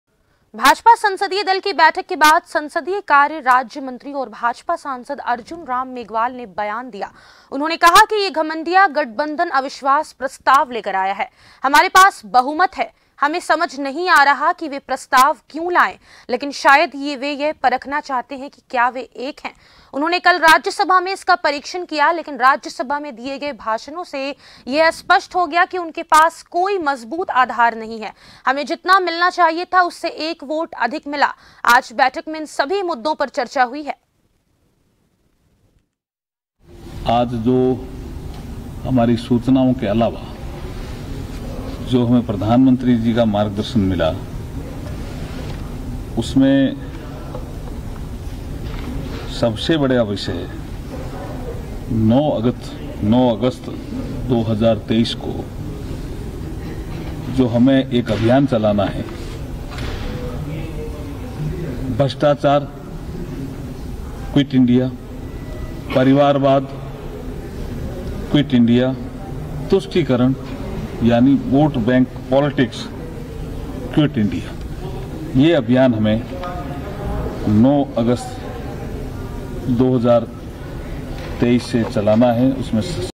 Delhi News | भाजपा संसदीय दल की बैठक आयोजित, सांसद अर्जुन राम मेघवाल ने दिया बयान | JAN TV
Watch JAN TV on :
Tata Play DTH : 1185
Airtel DTH: 355
JIO Fiber: 1384

News Credit-MKP